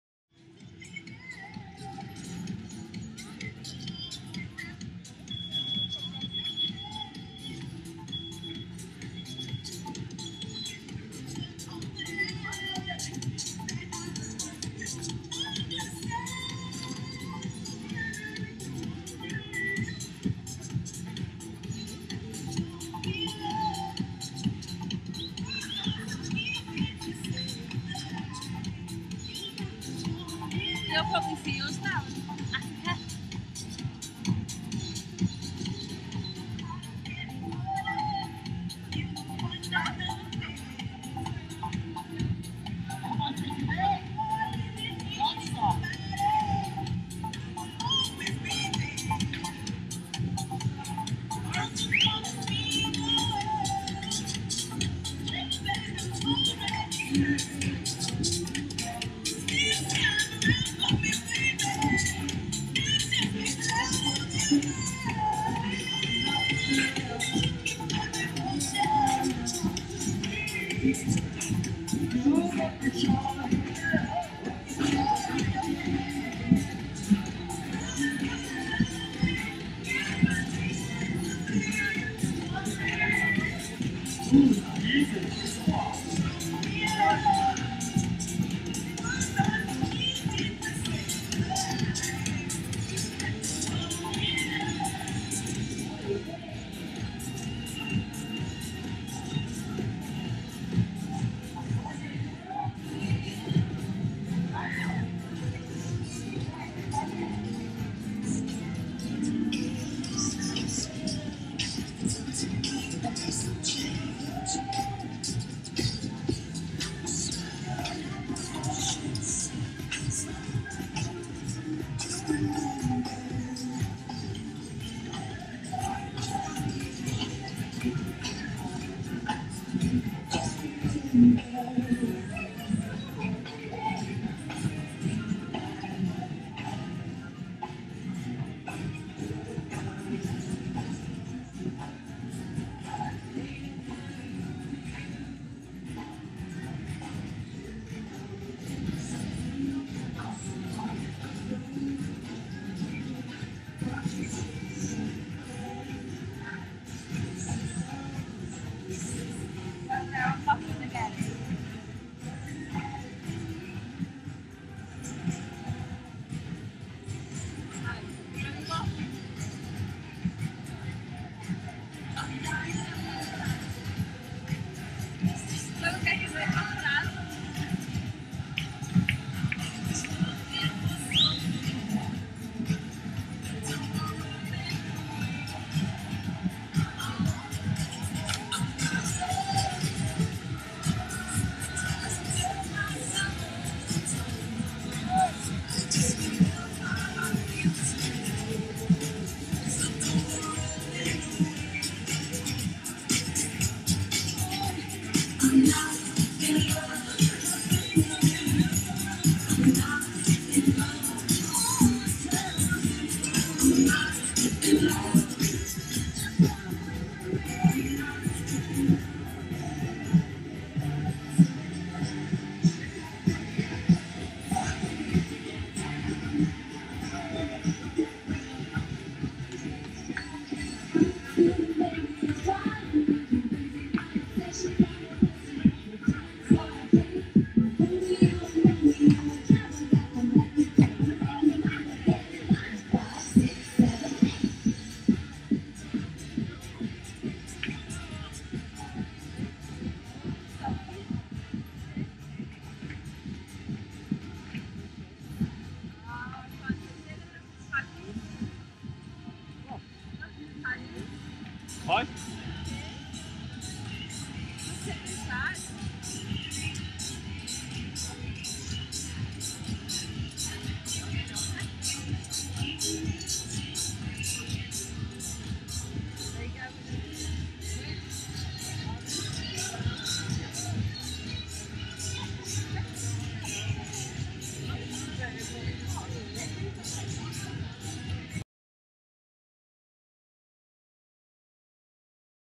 Hisaronu, Turkey at Night Sept 2019
Please hit the Thumbs Up button if you like this video.

Part of our Nightly walk through Hisaronu, heading for the Fez Bar.